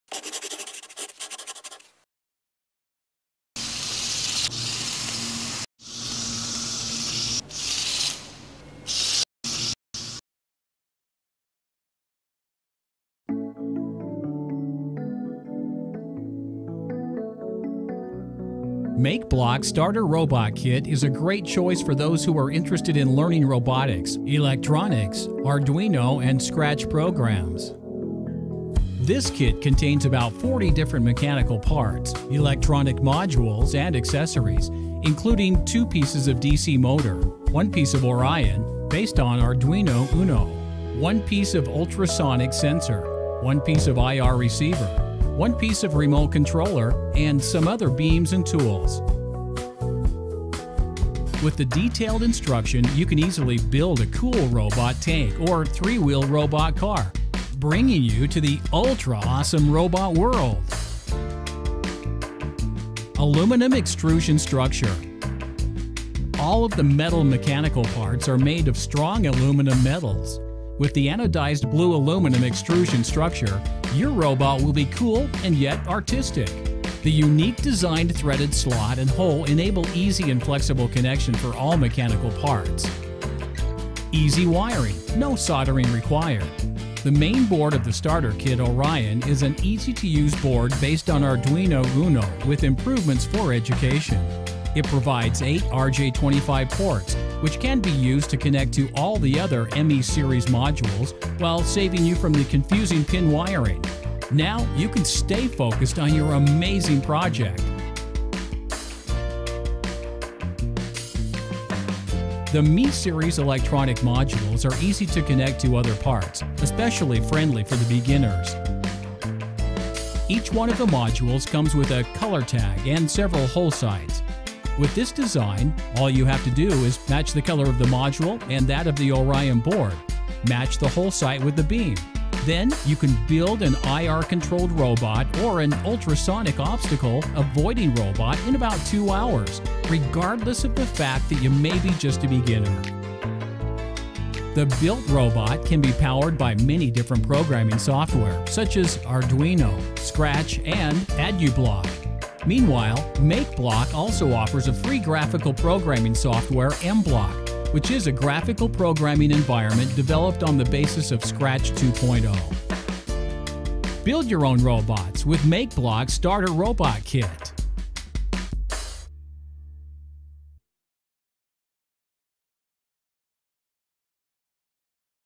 Makeblock Starter Robot Kit, Your First Step to Arduino, Sratch, Electronics and More!
Before mDrawbot and mBot, Starter Robot kit is the top lover by makers! There are multiple videos showcase online, find this one from official  Makeblock!